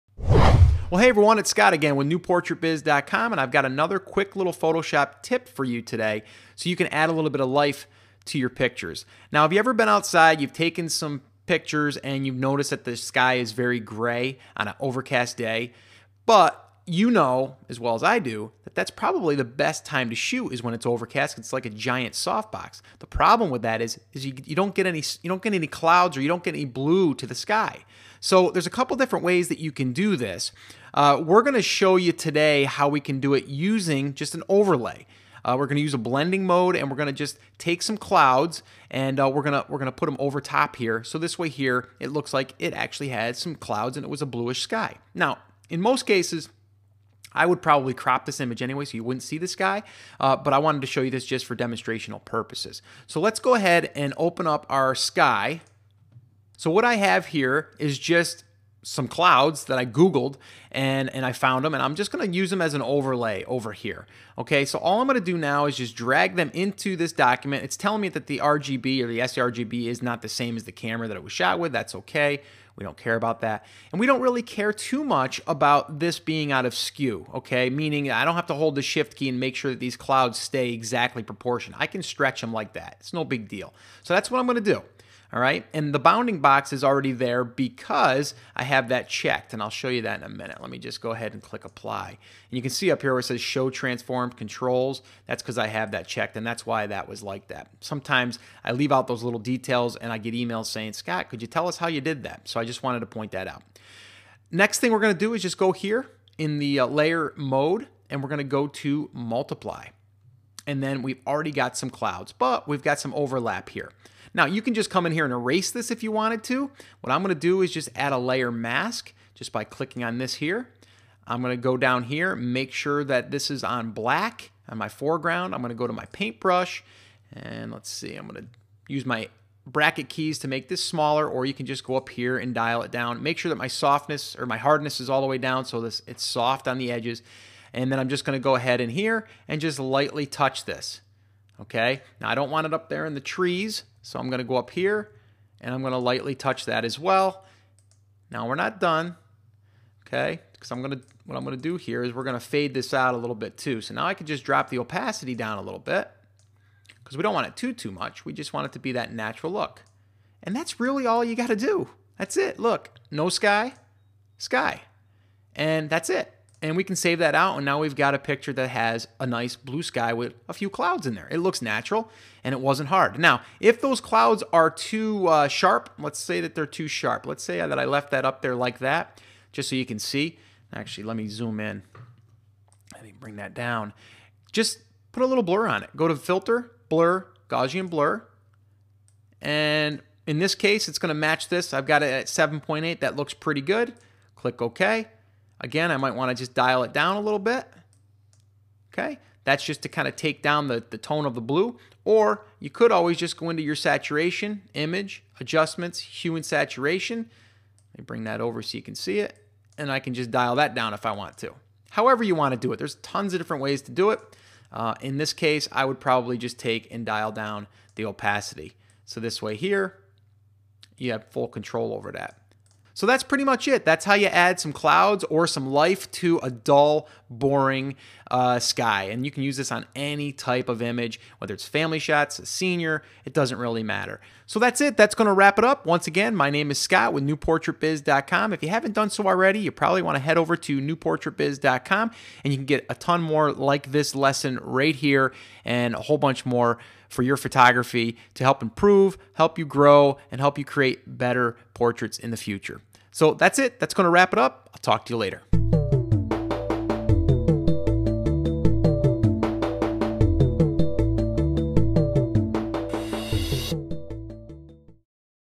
How To Add Clouds And Blue Sky To Digital Image - Photoshop Tutorial
- In this Photoshop Tutorial you'll learn how to turn an overcast sky into a blue sky with clouds. The basic technique is using an image of existing clouds and applying the multiply blending mode. This will bring the image to life and give it some true color.